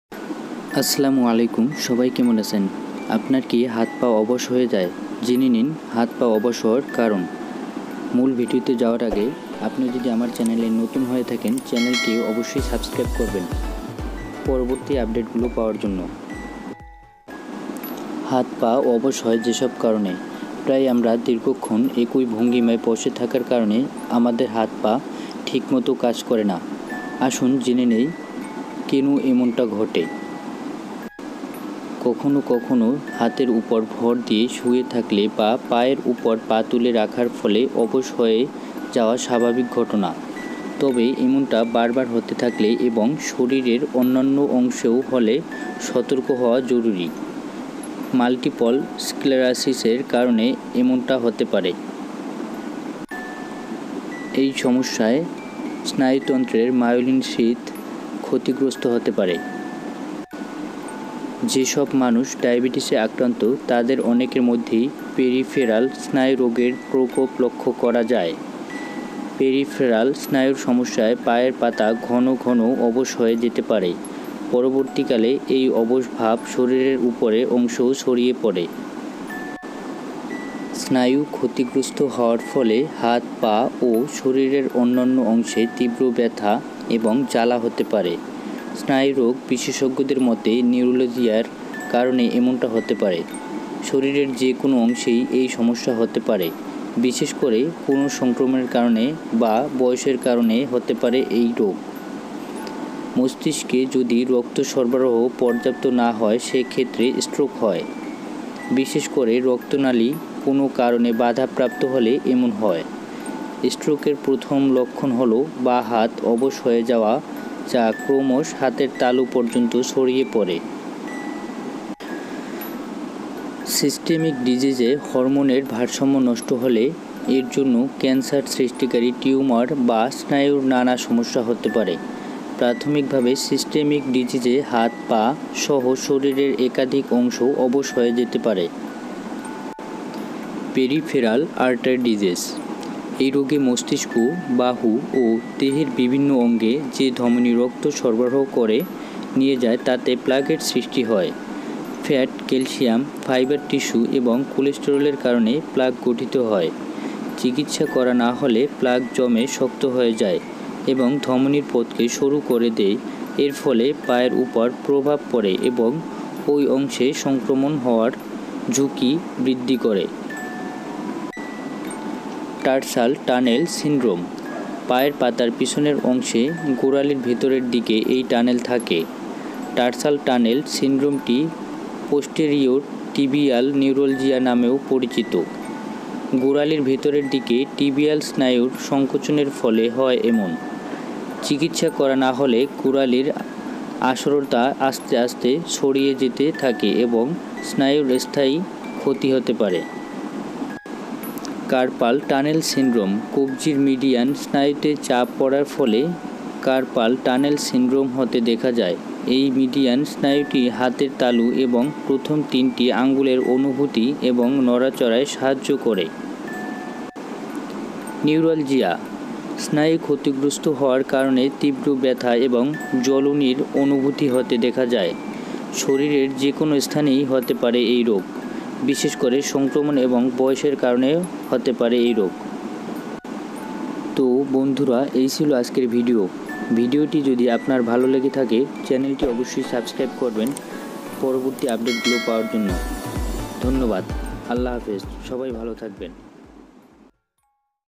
হাত পা অবশ হয় যেসব কারণে | জেনে নিন হাত পা অবশের কারন | why Disobedient hand and leg
মস্তিষ্কে যদি রক্ত সরবরাহ পর্যাপ্ত না হয় সে ক্ষেত্রে স্ট্রোক হয়। বিশেষ করে রক্তনালী কোনো কারণে বাধাপ্রাপ্ত হলে এমন হয়। স্ট্রোকের প্রথম লক্ষণ হলো বাঁ হাত অবশ হয়ে যাওয়া যা ক্রমশ হাতের তালু পর্যন্ত ছড়িয়ে পরে।

সিস্টেমিক ডিজিজে হরমোনের ভারসাম্য নষ্ট হলে, এর জন্য ক্যান্সার সৃষ্টিকারী টিউমার বা স্নায়ুর নানা সমস্যা হতে পারে। প্রাথমিক ভাবে সিস্টেমিক ডিজিজে হাত, পা-সহ শরীরের একাধিক অংশ অবশ হয়ে যেতে পারে।

creator : md mamun khan